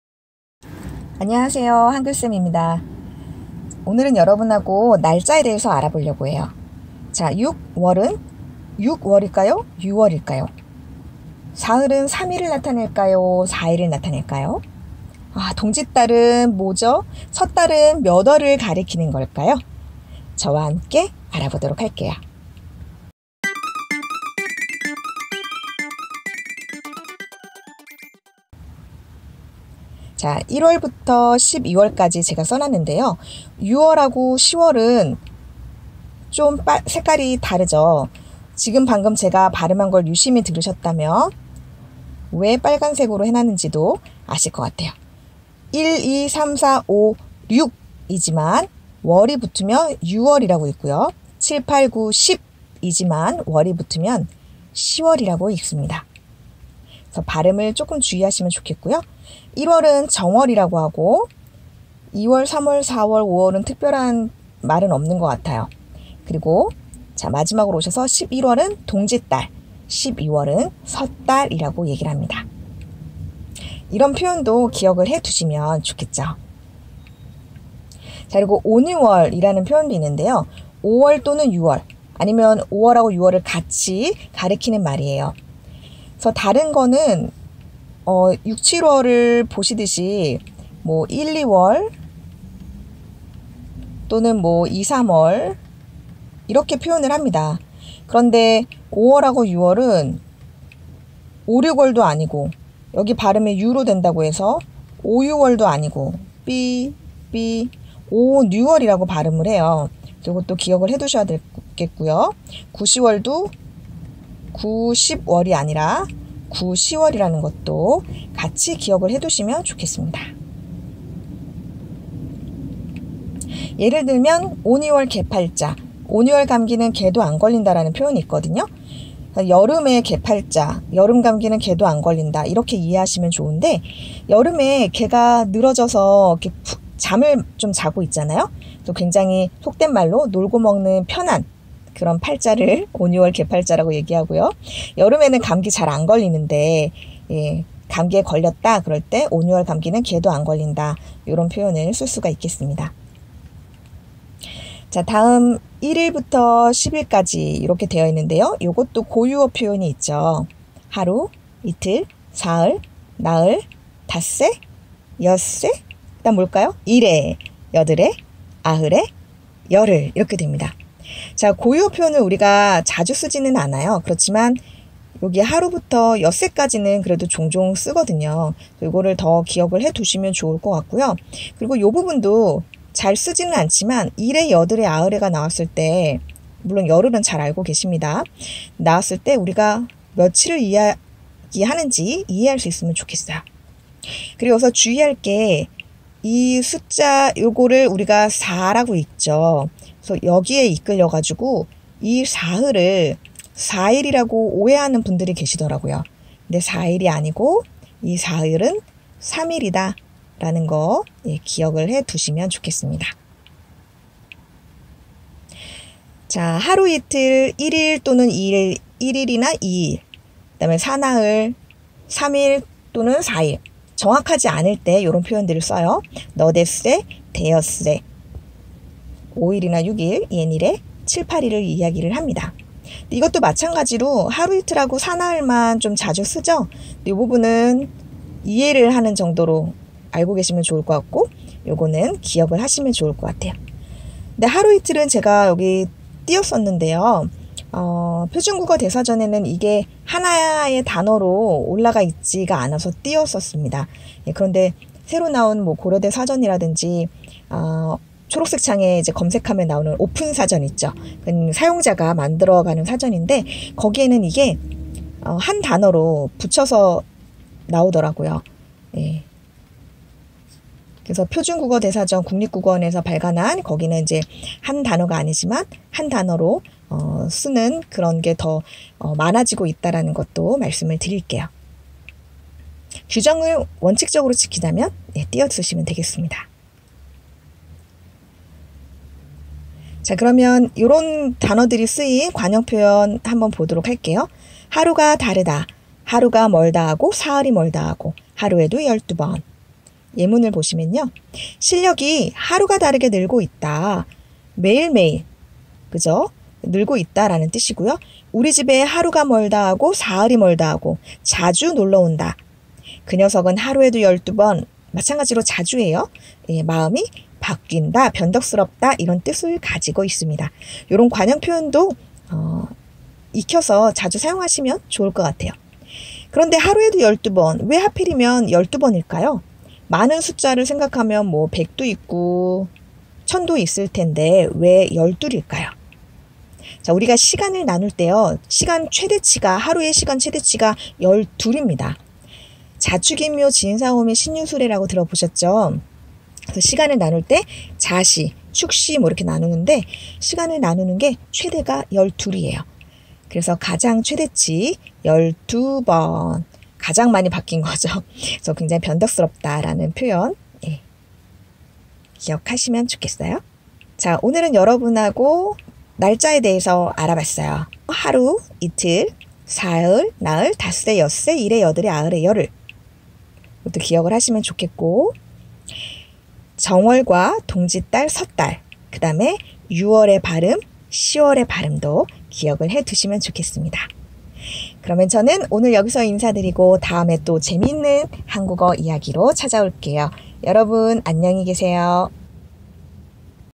[한국어 어휘 Korean Vocabulary - 날짜 dates] 사흘은 3일? 4일?
[한국어 어휘]  날짜
Q.  6월은 발음이 [육월]일까? [유월]일까?
Q. 사흘은 3일일까? 4일일까?
Q. 동짓달과 섣달은 몇 월일까?
궁금하다면!! 한글샘과 함께 알아봐요. ^^

[Korean Vocabulary] Dates
Q. What is the pronunciation  '6월(month)' ?
Q. What is the meaning of '사흘[sa-heul]'?
Q. Do you know the words '동짓달[dong-jit-tal]' and '섣달[tal]'?